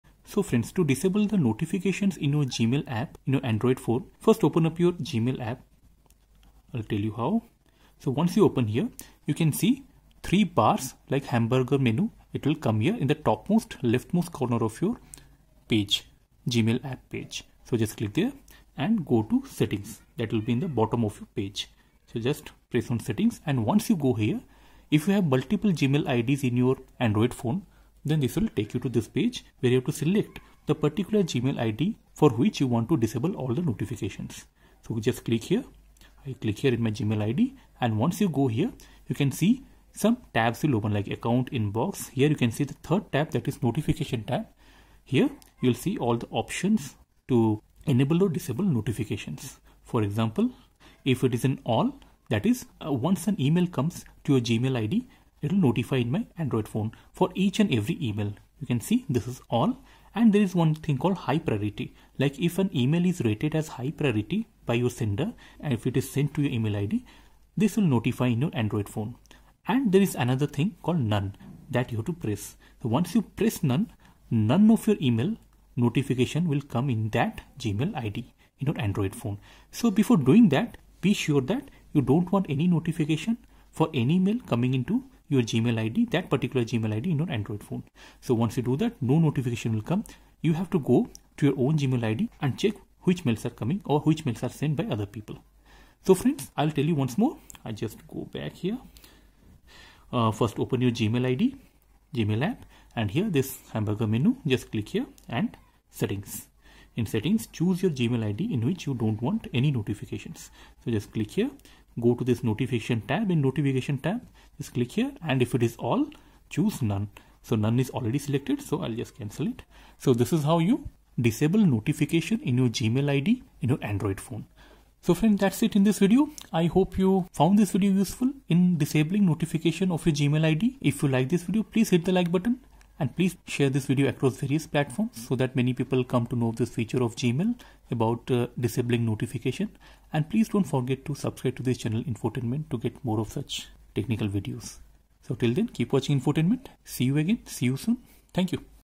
How to Enable or Disable Gmail Notifications on Android
How to Disable Gmail Notifications on Android
On your Android phone or tablet, open the Gmail app for  how to turn on gmail notifications on phone
In the top left, tap Menu .
Tap Settings.
Select your account.
Tap Notifications select All / None.